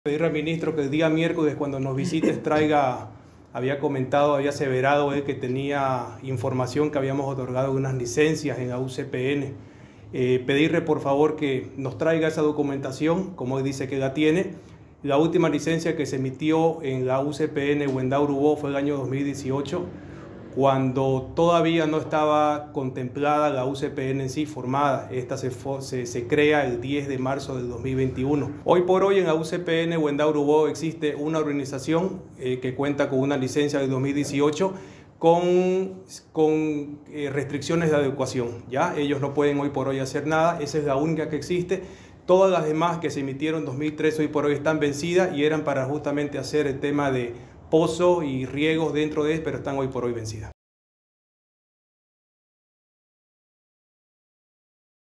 Gobernación aclara que no emitió licencias para urbanizar en UCPN Guendá-Urubó
_Aclaración. La anterior gestión emitió una licencia para una urbanización en 2018, antes de crear la UCPN; sin embargo, esta se encuentra con restricciones de adecuación y no pueden hacer nada en el lugar_

*Santa Cruz, 28 de agoto 2023* La Gobernación desmintió al Gobierno, quién acusó a la gestión del gobernador Luis Fernando Camacho de entregar licencias a urbanistas en la Unidad de Conservación del Patrimonio Natural (UCPN) Guendá-Urubó y señaló que en la gestión 2018 la gestión pasada entregó un certificado en la zona.

Pablo Sauto, secretario de Desarrollo Sostenible y Medio Ambiente, indicó que dentro de las 44.700 hectáreas de la UCPN Guendá-Urubó solo existe parte de una urbanización; sin embargo, aclaró que la licencia data del año 2018, antes de la creación de la UCPN (10 de marzo 2021).

“Esta urbanización está en periodo de adecuación, no puede hacer ninguna intervención en el lugar hasta que no tengamos el plan de manejo de la UCPN y poder dar los lineamientos respectivos. Todas las licencias emitidas en 2013 hoy están vencidas y eran para hacer pozos y riego dentro del área, pero están vencidas”, aclaró Sauto.

Agregó que el tercer cambio que se realizó al diseño de la carretera ahora pasa más cerca de esa urbanización, y como el tramo 2 como está diseñado pone en riesgo la zona de recarga acuífera, ratificó que la Gobernación no permitirá la construcción sobre este sitio.